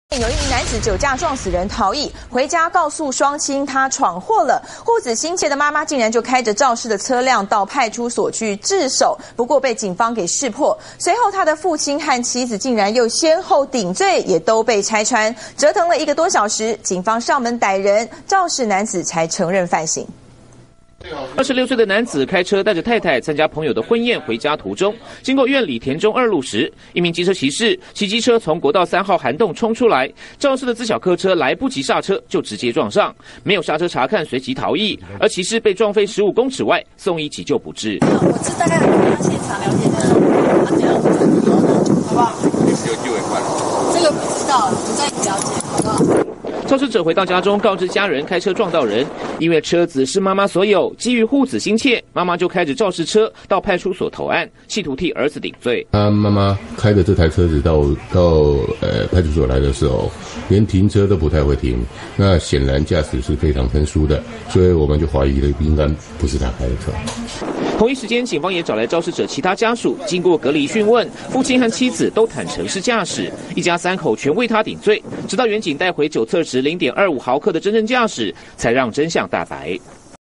中視新聞》酒駕男撞死人逃逸 父母、妻爭相頂罪
中視新聞》苗栗苑裡1名男子酒駕撞死人逃逸，回家告訴雙親闖禍了，護子心切的媽媽開著肇事車到派出所自首，被警方識破，隨後父親和妻子竟然又先後頂罪也被拆穿，折騰了1個多小時警方上門逮人，肇事男子才承認犯行。